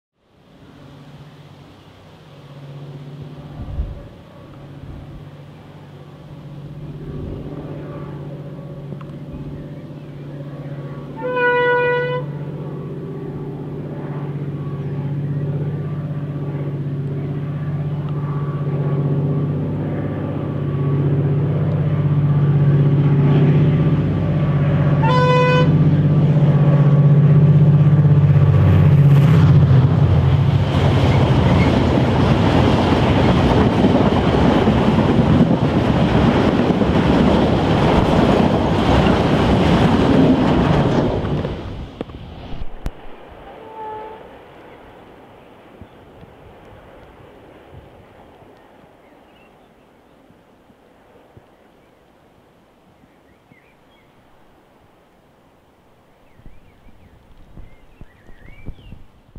071 on Waterford-Westport empty timber train west of Athlone 17-May-2013
Freshly repainted class leader 071 heads a Waterford-Westport empty timber train west of Athlone 17-May-2013.